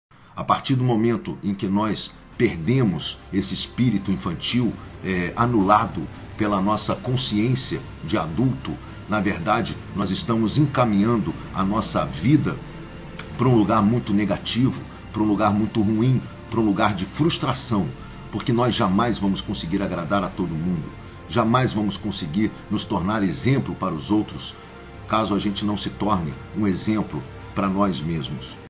Não perca seu espírito infantil!
Quando perdemos nosso espírito infantil que é anulado por nossa consciência adulta, nós encaminhamos nossa vida para um lugar negativo! Não deixe isso acontecer!

⚠️ Participe do Workshop Gratuito: As 7 Riquezas do Universo e a Força do Amor-Próprio!

Clique agora no link e garanta já sua inscrição